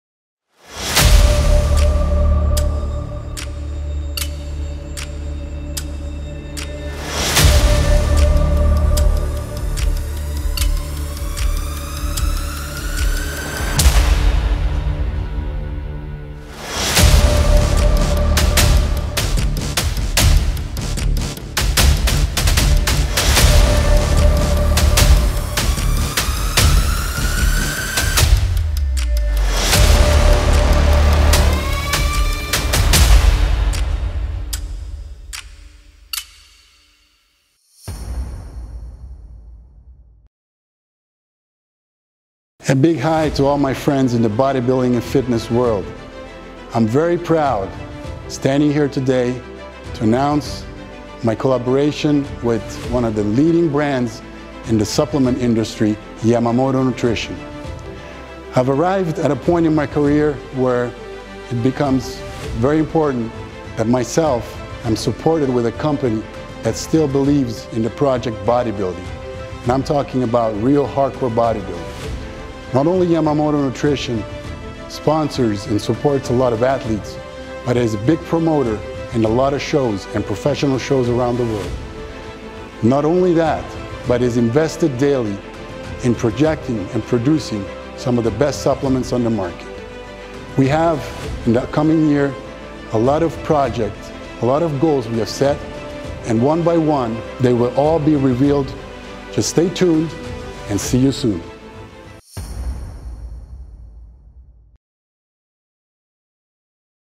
The most successful bodybuilding coach | Patrick Tour
Welcome to Yamamoto Nutrition's team to one of the most famous European trainers, creator of sarcoplasma stimulation training and the top speaker on everything that matters for bodybuilding 😈🌟